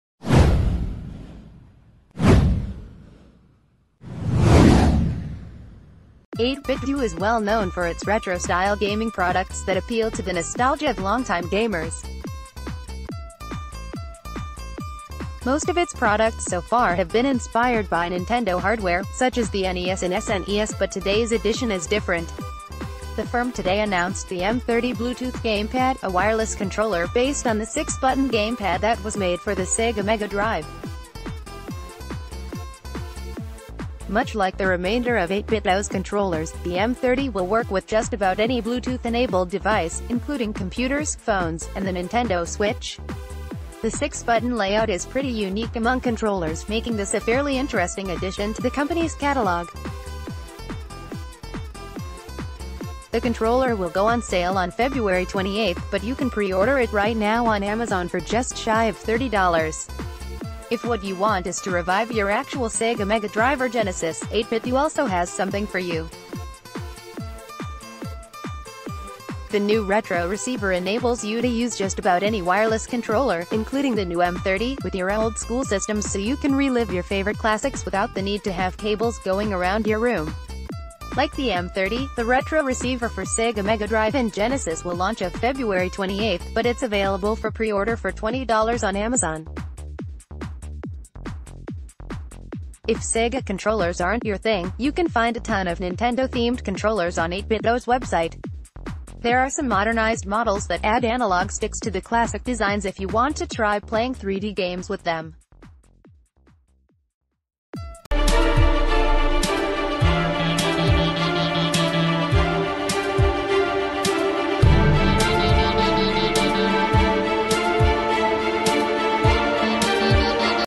8Bitdo's newest retro controller is aimed at SEGA fans
8Bitdo's newest retro controller is aimed at SEGA fans
8Bitdo's newest retro controller is aimed at SEGA fans

8Bitdo is well known for its retro-style gaming products that appeal to the nostalgia of long-time gamers. Most of its products so far have been inspired by Nintendo hardware, such as the NES and SNES, but today's addition is different. The firm today announced the M30 Bluetooth gamepad, a wireless controller based on the six-button gamepad that was made for the SEGA Mega Drive. Much like the remainder of 8Bitdo's controllers, the M30 will work with just about any Bluetooth-enabled device, inclu...